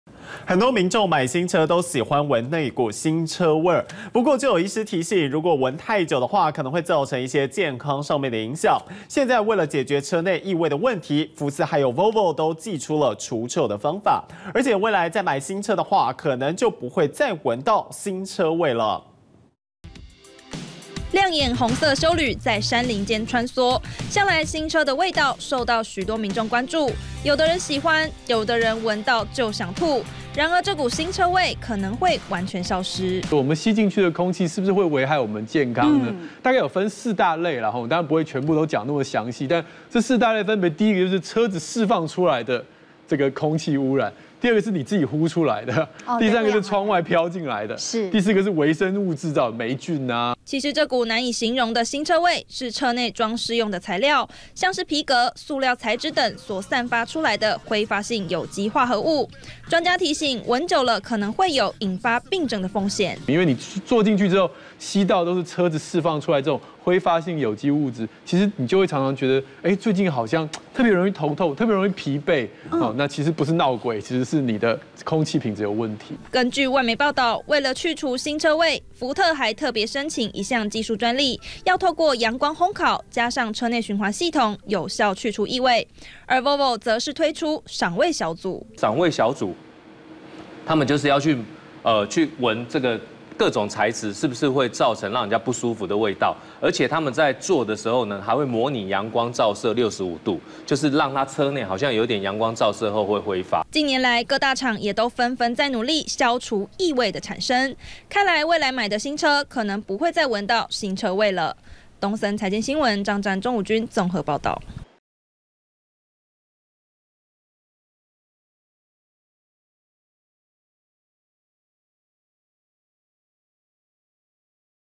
就愛聞新車那股味？ 未來「新車味」恐消失
很多民眾買新車，都喜歡聞那股「新車味」，不過就有醫師提醒，如果聞太久，可能會造成一些健康上面的影響，現在為了解決車內異味的問題，福斯和Volvo都祭出除臭方法，而且未來買新車，可能就不會再聞到新車味了！